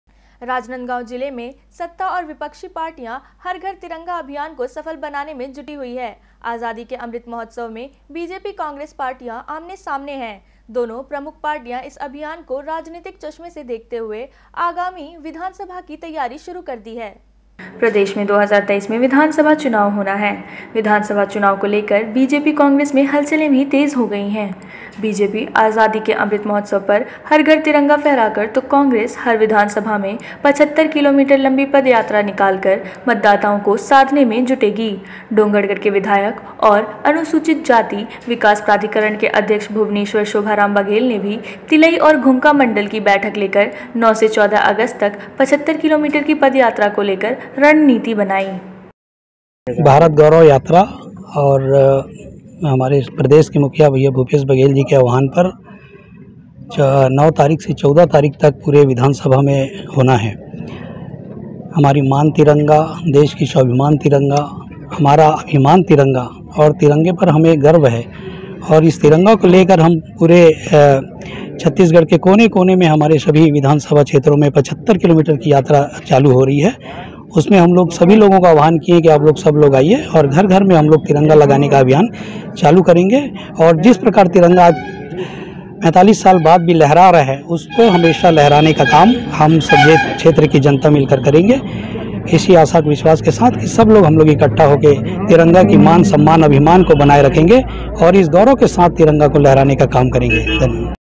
Rajnadgaon ' Her Ghar Tiranga' अभियान को सफल बनाने के लिए BJP - Congressहोंगे आमने-सामने@IndianNewsMPCG
Indian News :  राजनांदगांव जिले में सत्ता और विपक्षी पार्टियां हर घर तिरंगा अभियान को सफल बनाने में जुटी हुईं हैं। आजादी के अमृत महोत्सव में बीजेपी-कांग्रेस पार्टियां
आमने-सामने हैं। दोनों प्रमुख पार्टियां इस अभियान को राजनीतिक चश्मे से देखते हुए आगामी विधानसभा की तैयारी शुरू कर दी है।

 प्रदेश में 2023 में विधानसभा चुनाव होना है। विधानसभा चुनाव को लेकर बीजेपी-कांग्रेस में हलचलें भी तेज हो गईं हैं। बीजेपी आजादी के अमृत महोत्सव पर
हर घर तिरंगा फहराकर तो कांग्रेस हर विधानसभा में 75 किलोमीटर लंबी पदयात्रा निकालकर मतदाताओं को साधने में जुटेगी। डोंगरगढ़ के विधायक और अनुसूचित जाति विकास प्राधिकरण के अध्यक्ष भुवनेश्वर शोभाराम बघेल ने भी तिलई और घुमका मंडल की बैठक लेकर 9 से 14 अगस्त तक 75 किलोमीटर की पदयात्रा को लेकर